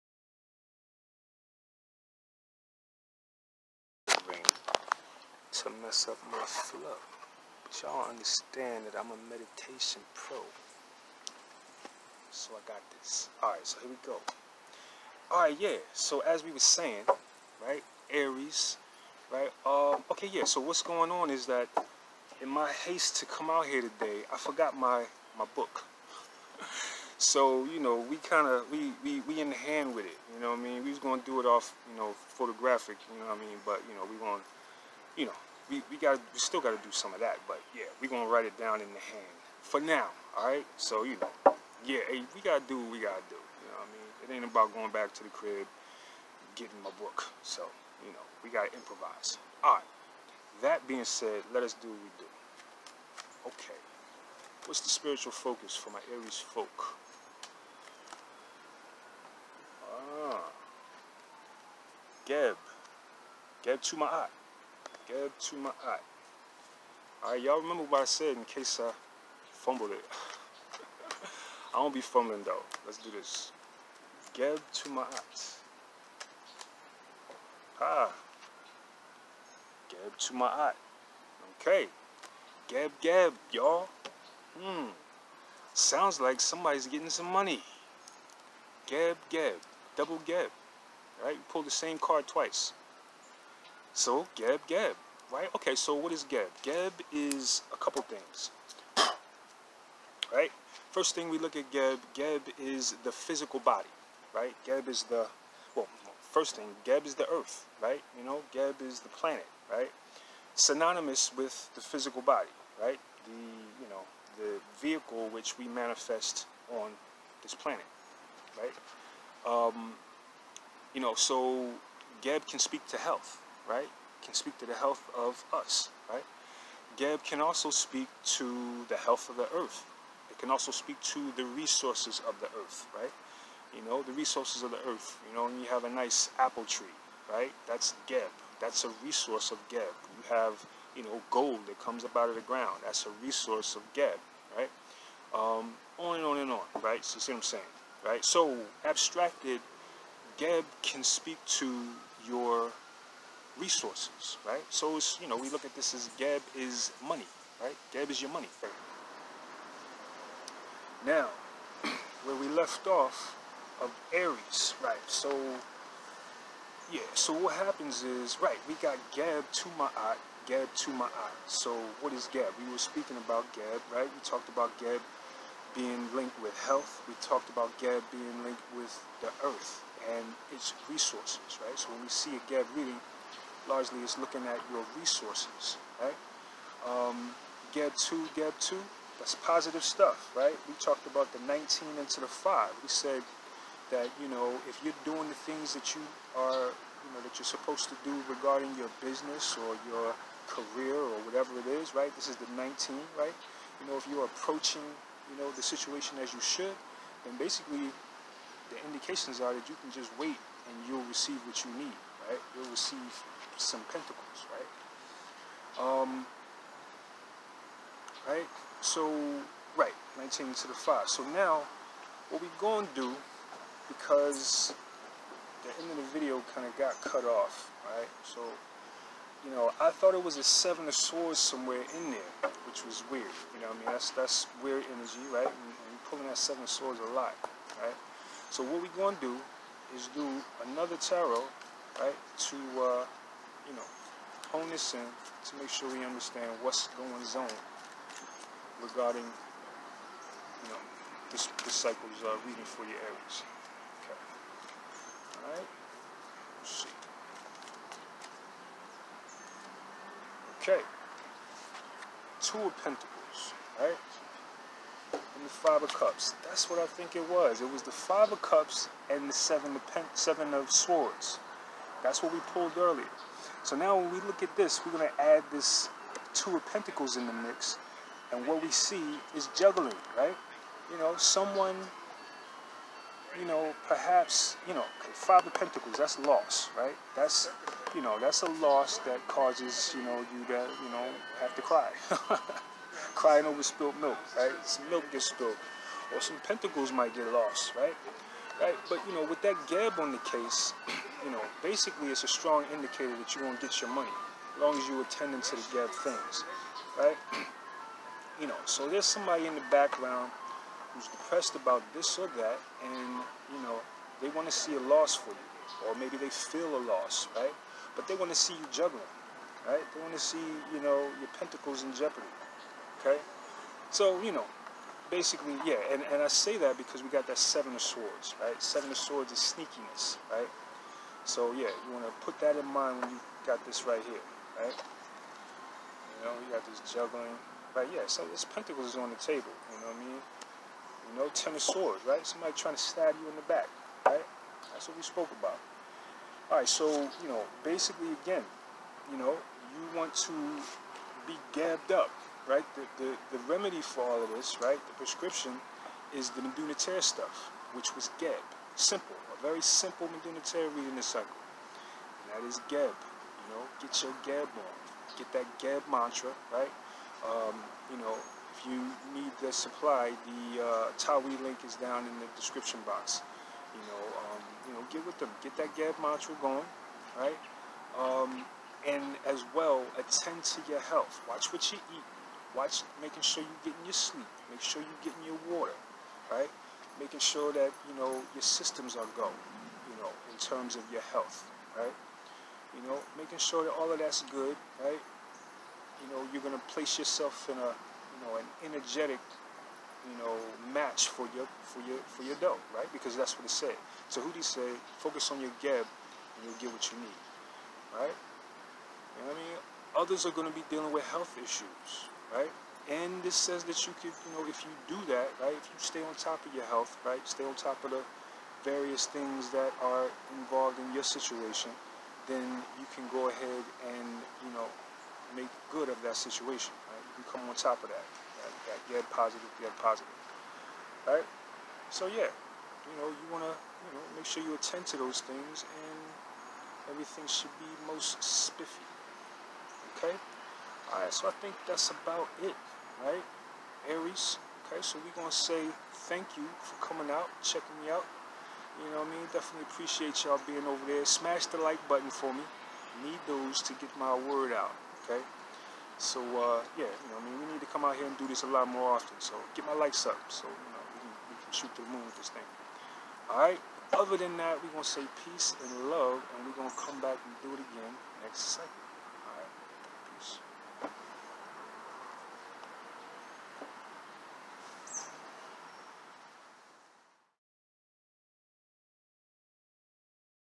August Aries Reading: Eyelids up! Pentacles Watch!
This is our monthly New Moon cycle, Overall collective Spiritual Focus reading for Aries. If the reading resonates leave a like or comment.

If you appreciate this content show ya love.

See also:
I Ching Divination: The Beginner's Guide by Saanu aqa

Incredible Omen Discoveries re-presented as NFT's.